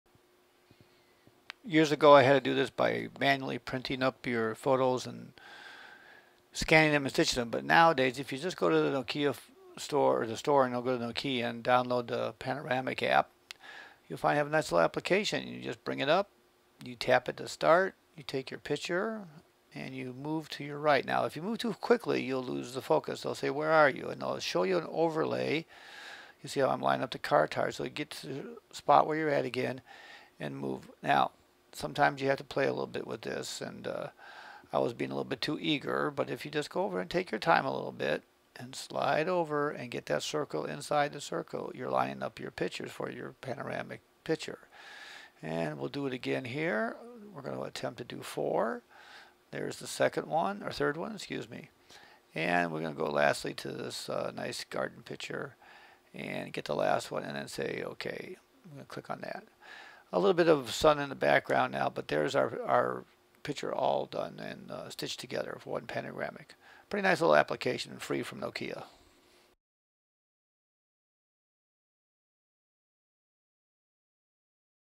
Windows 8: Nokia Panoramic App Picture Stitching
I used to have to take printed pictures, scan them in and then run a program where I had to manually align pictures together to make a panoramic scene.

Man, has it gotten easier!!  Just go to the Windows Phone Store and under Nokia Apps find the Panoramic App.  Install it and panoramic scences are just a few clicks away!